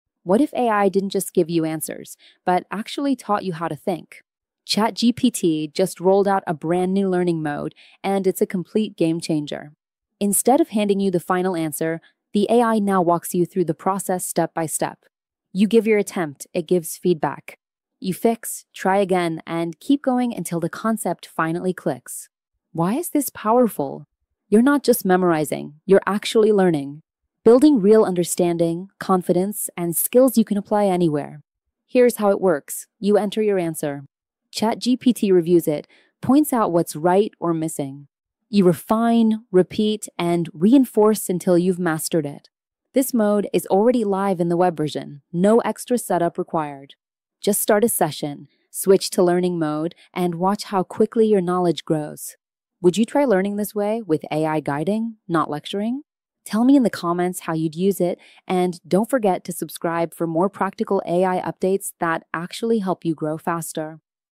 The SHOCKING TRUTH About ChatGPT Learning Mode AI Nobody Tells You
Discover ChatGPT’s new Learning Mode — an interactive way to learn step by step with instant feedback instead of memorizing answers. Try it now in the web version and upgrade how you study with AI!

ChatGPT just released Learning Mode — a feature that turns your AI into a personal tutor. Instead of giving you answers, it guides you through the process, corrects your mistakes, and helps you truly understand the topic. Available now in the web version.

How would you use Learning Mode in your studies or work?